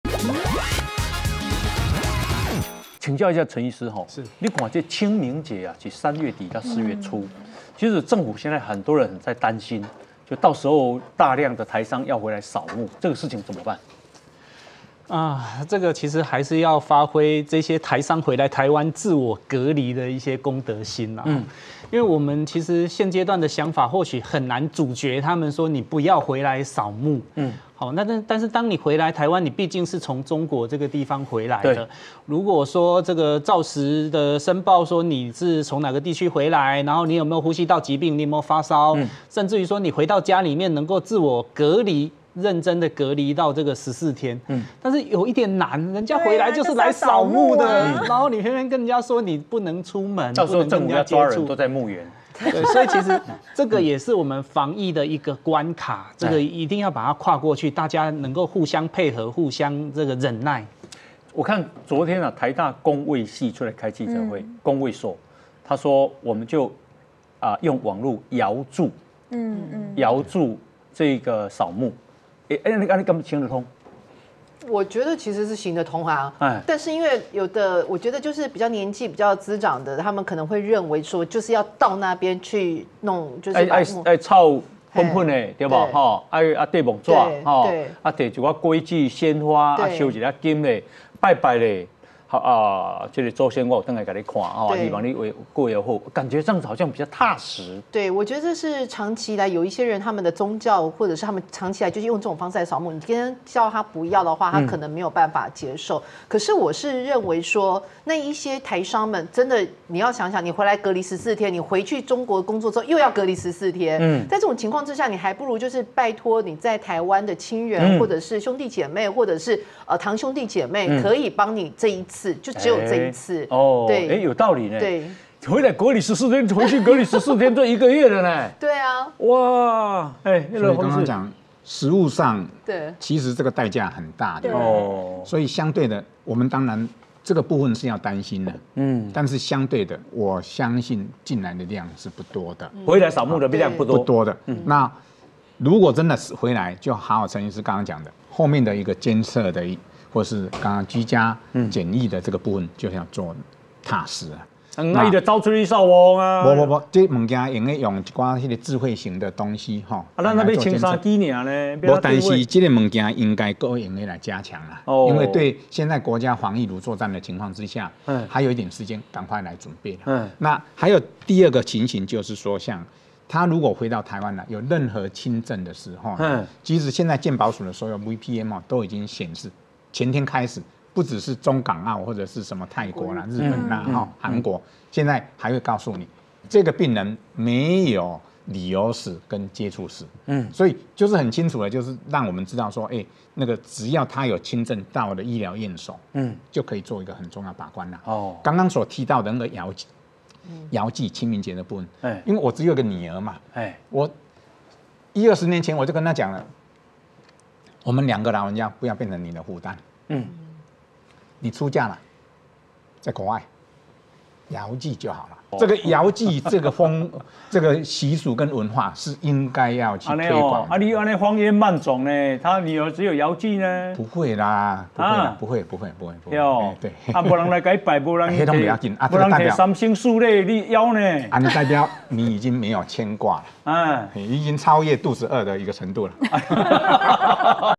【精華版】台商掃墓衝擊疫情？醫師：代價很大的！
從中國返台要先隔離14天，回中國又要隔離14天，這會影響台商選擇嗎？